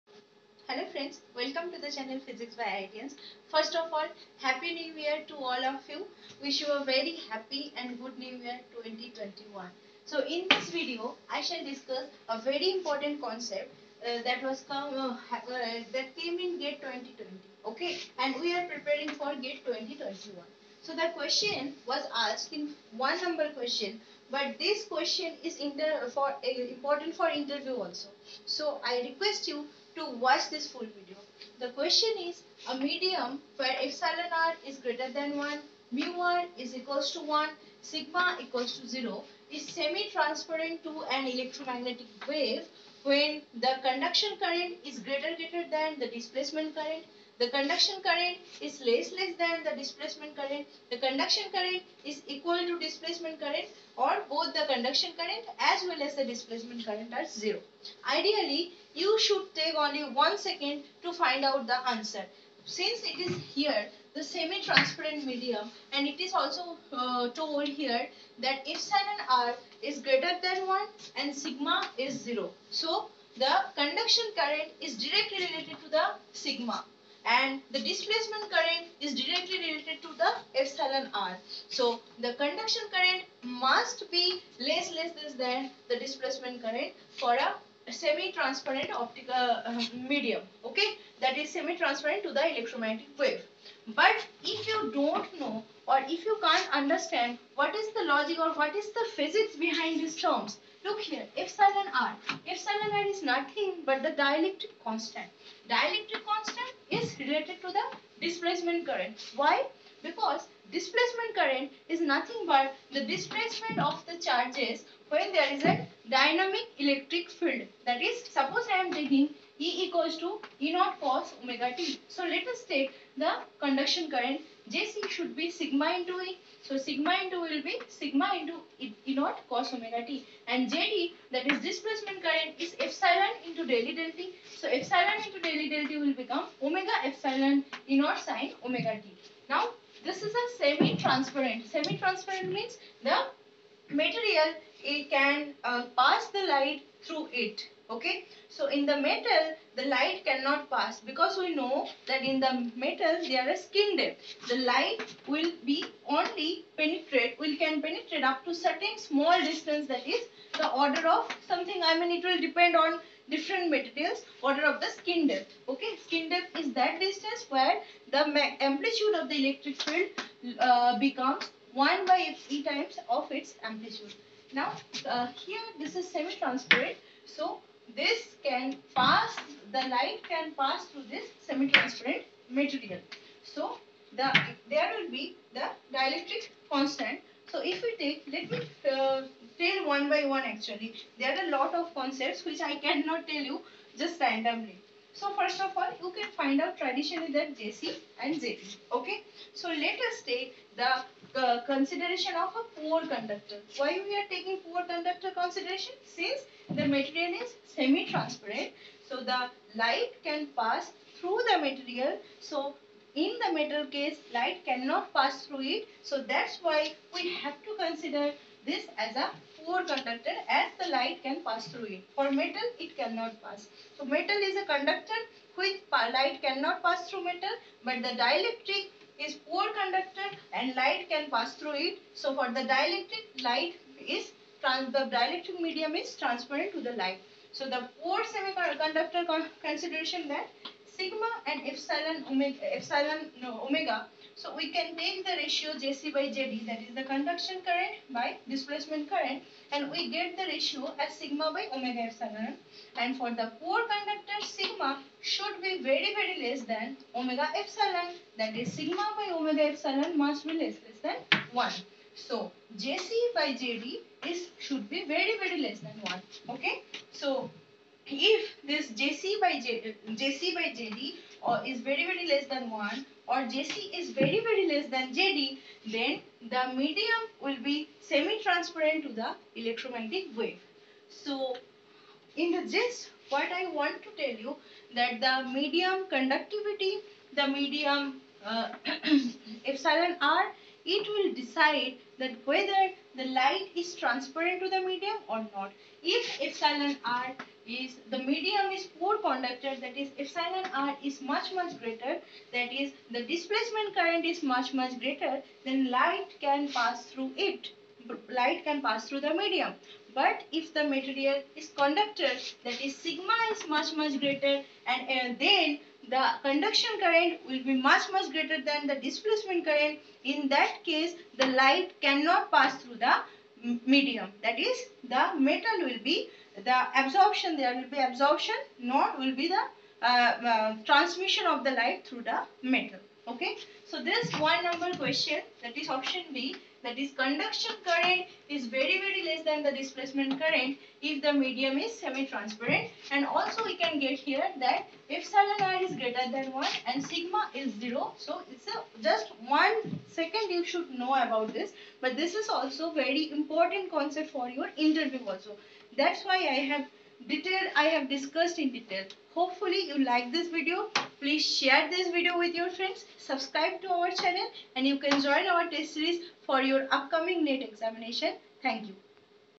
Electrodynamics👉 conduction current and displacement current 👉🏽Important for Physics Interviews
The content will help you in qualifying competitive exams and interviews for higher education in Physics
Important for competitive exams like CSIR NET GATE JEST BARC TIFR IIT JAM like exams.